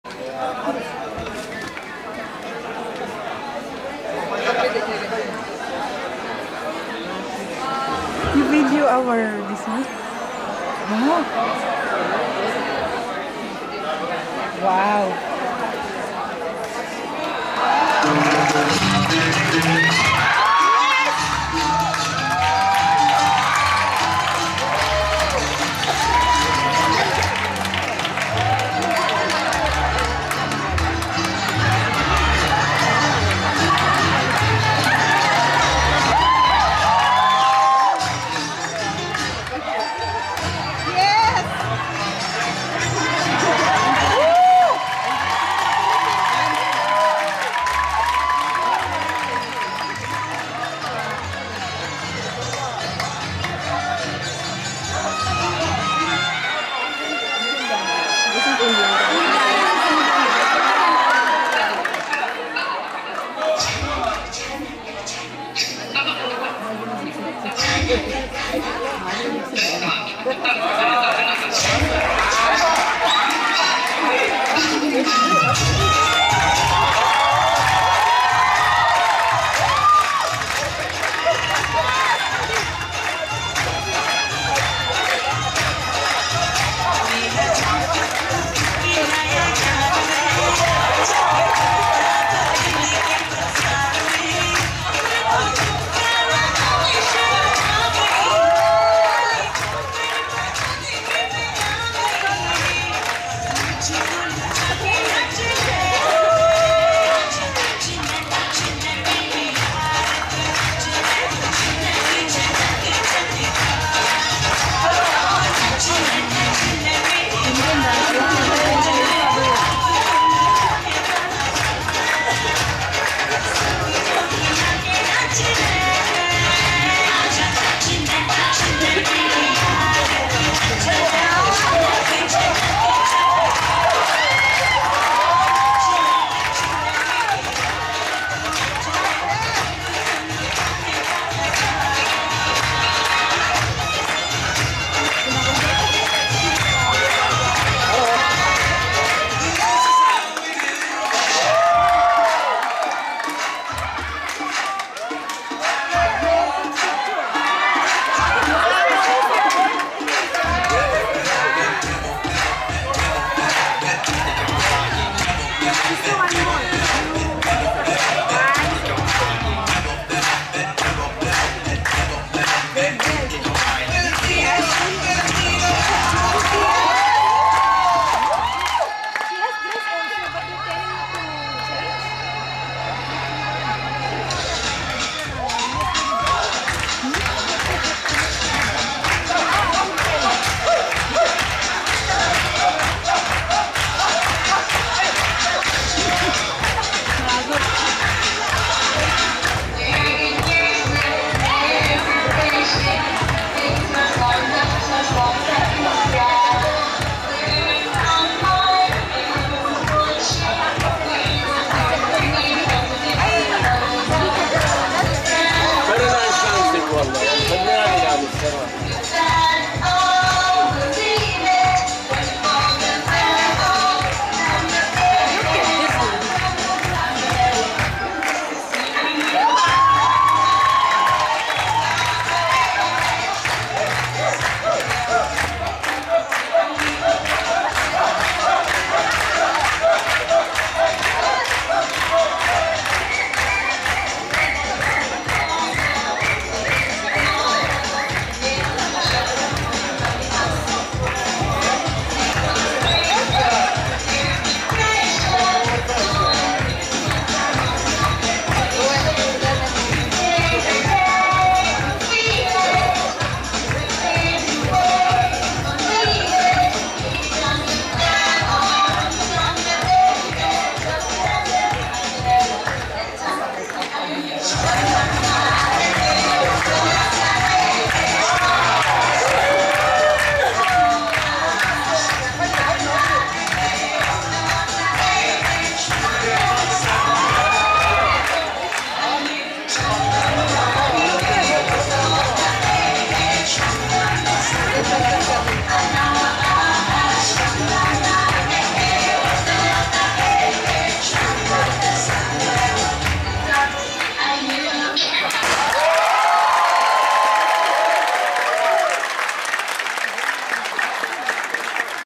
NURSES GOT TALENT(RHTC WARD 21 CARDIAC INTERVENTIONAL UNIT)
"Unity in diversity'. This is our official entry for the 2011 Nurses Day Celebration (last May 11,2011 at RH Main Auditorium).
In this Video:Diana Mendez-Choreographer,Charito Corcuera,Michelle Olitoquit,Alexander Lim-Music Mixer/Audio Visual Coordinator,Jonathan Lasin,Anu Paul,Seema V.,Rinta Elsy George,and Leni Biju(Costume Consultant)